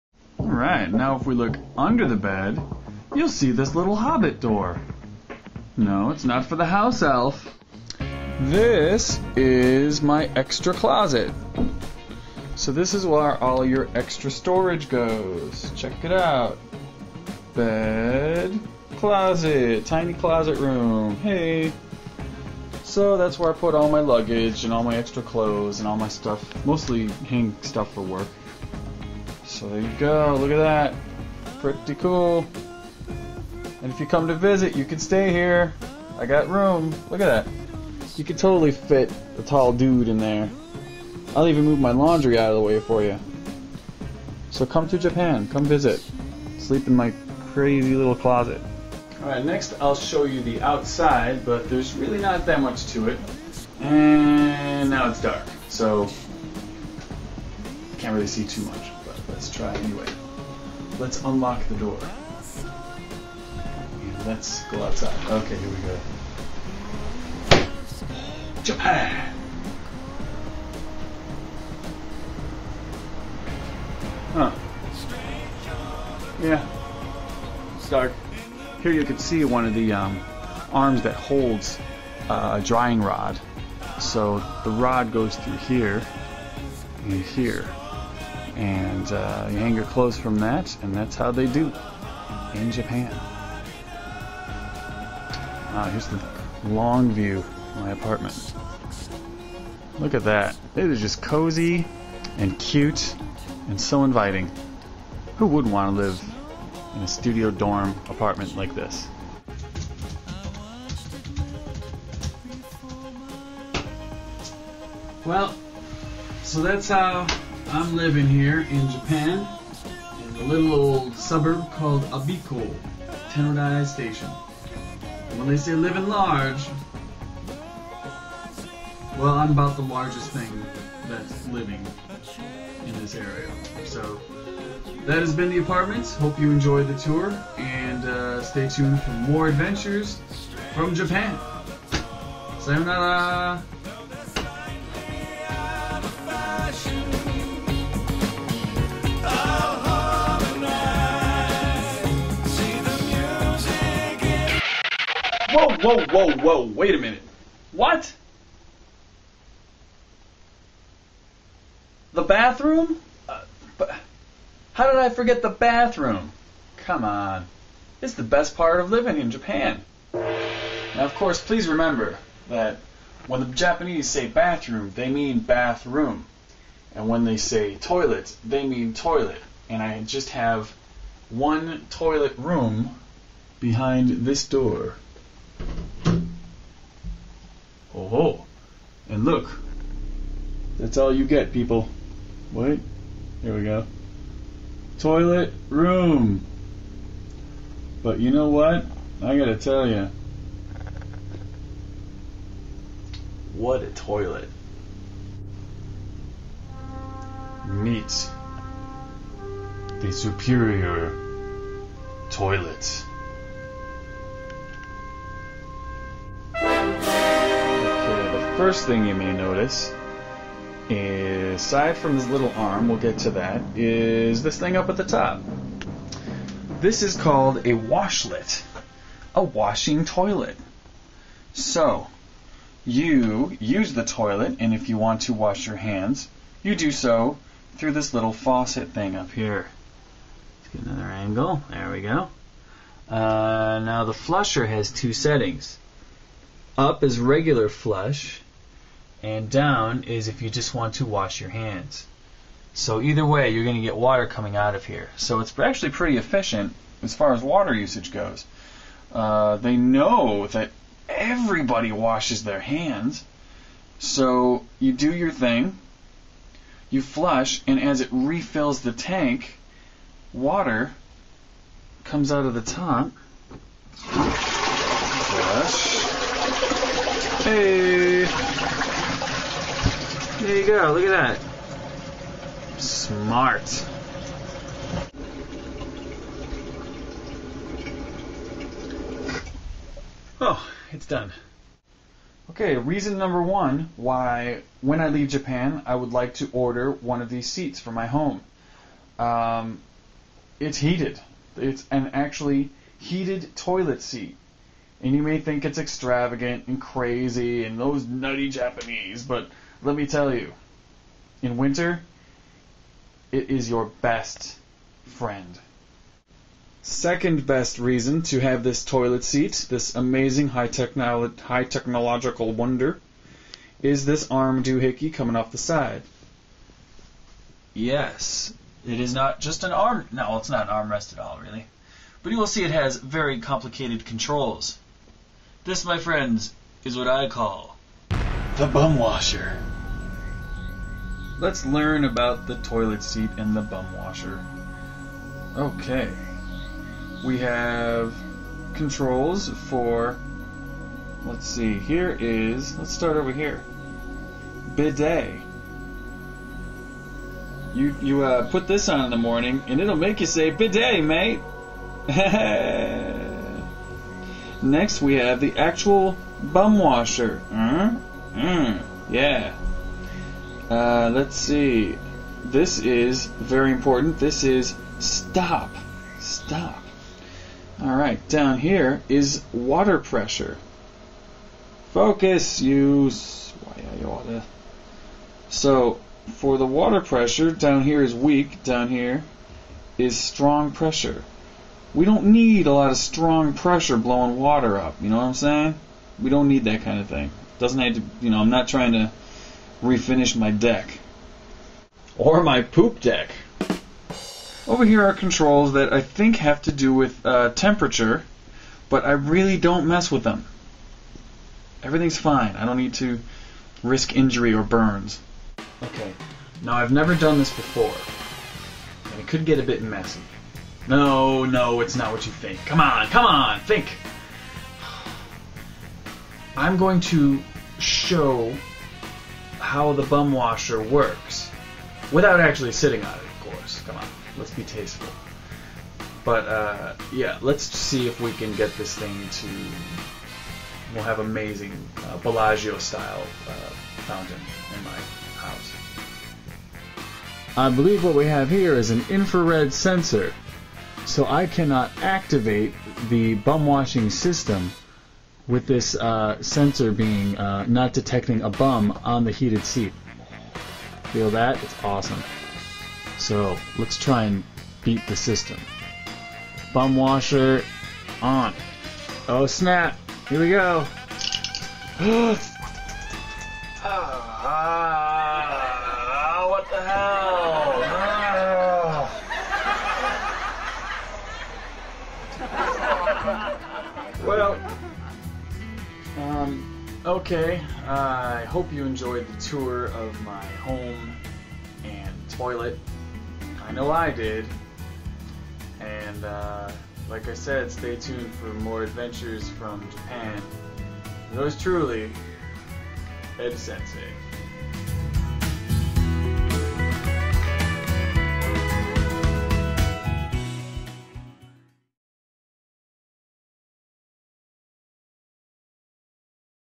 Apt 204: Moving to Abiko Part 4• December 6, 2008
After a great start in Asakusa, the gang of ALTs is broken up into groups and sent to different points of Tokyo to await a more permanent placement. Catherine, Ali and I were shipped out to Abiko together. Here we start the tour of my Japanese apartment.